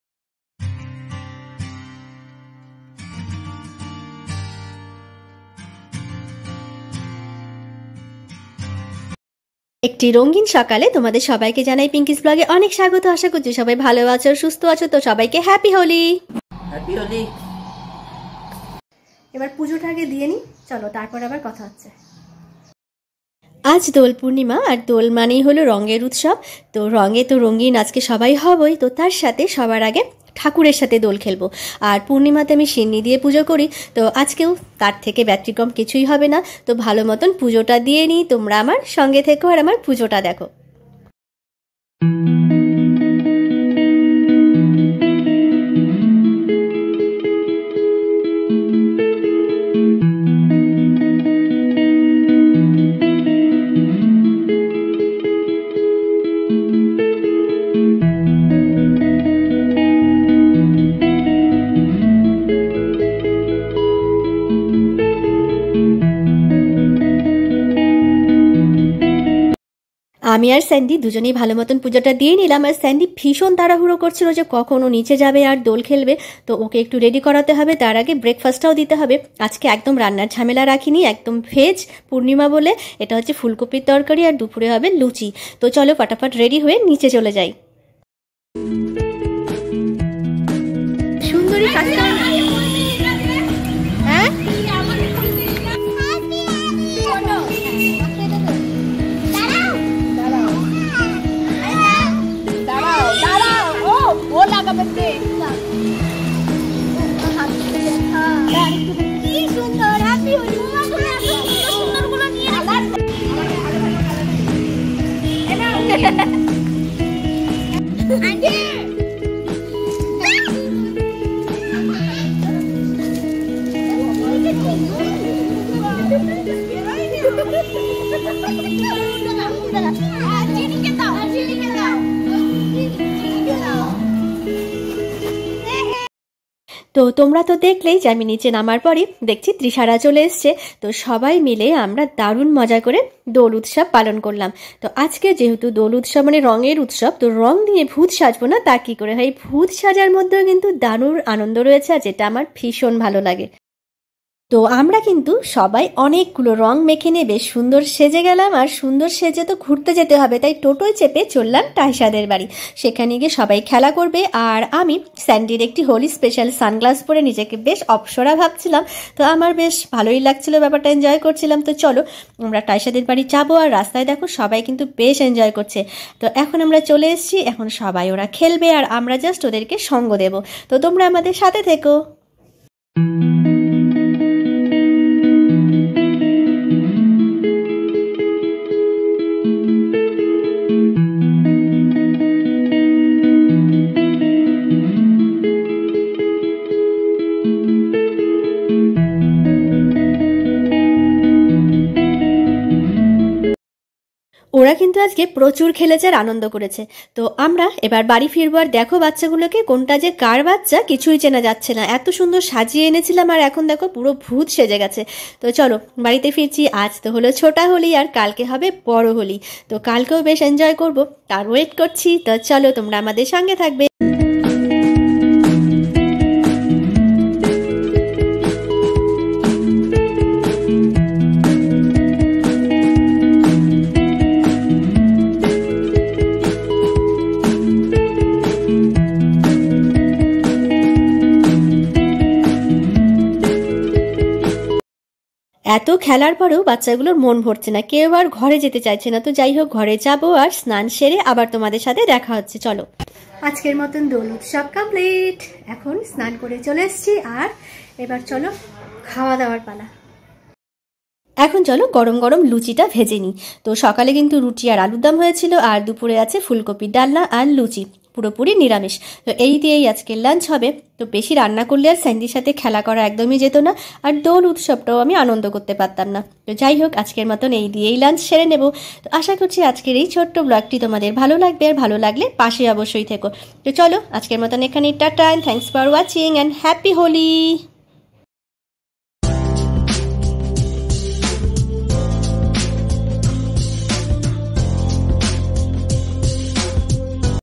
Ebarer Holi Evabei Katlo😎Happy Holi Everyone😍😎😍
মায়েদের সব আনন্দই সন্তানের জন্য Sandy র জন্যই দোলটা এত ভাল কাটল❤️happy holi to all of u😍😍❤️